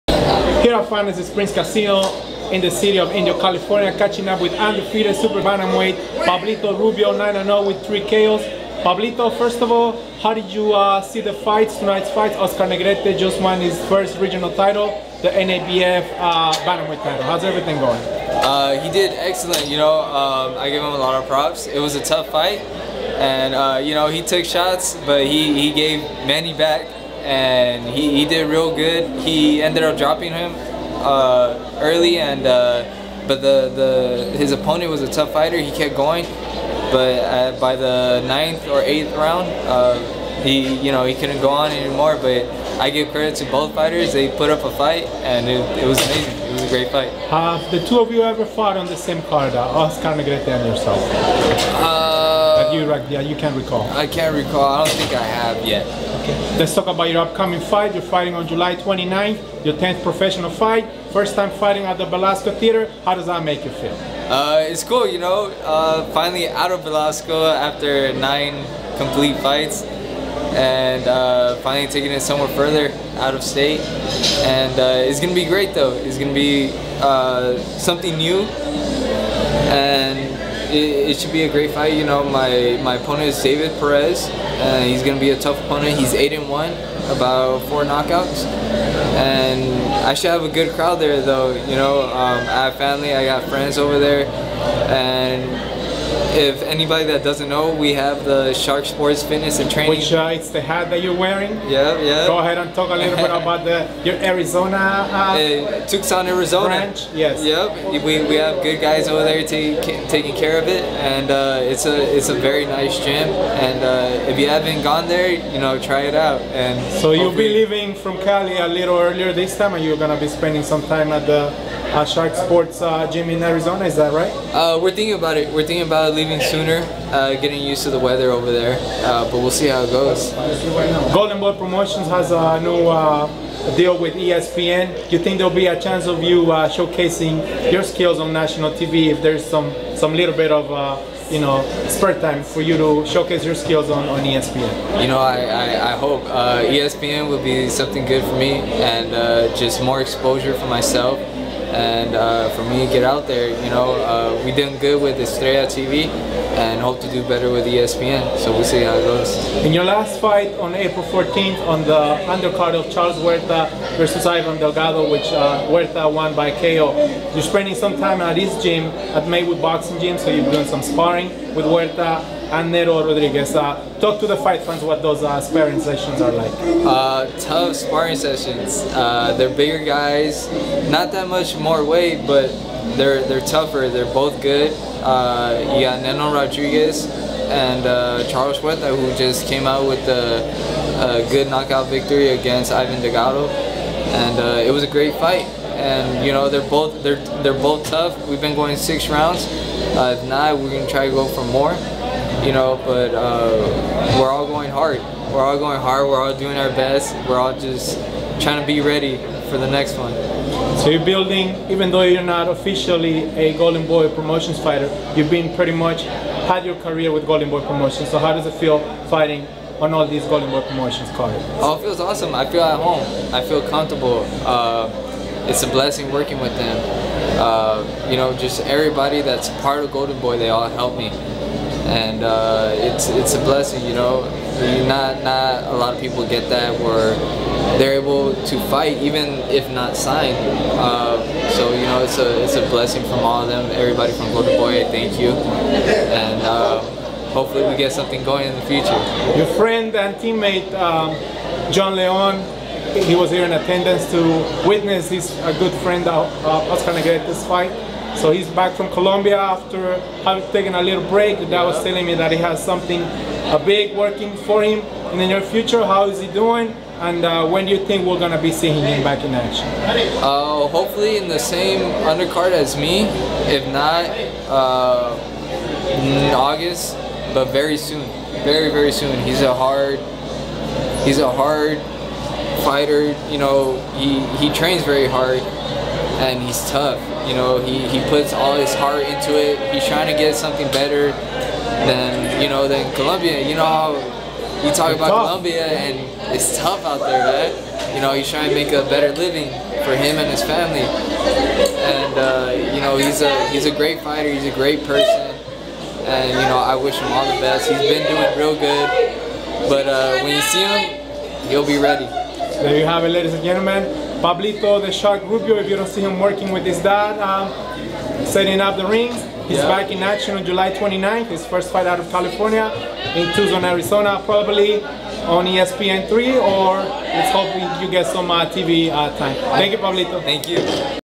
Pablo Rubio Jr.-Shark Sports Fitness and Training Gym and his 10th pro fight on July 29th.
Indio, Ca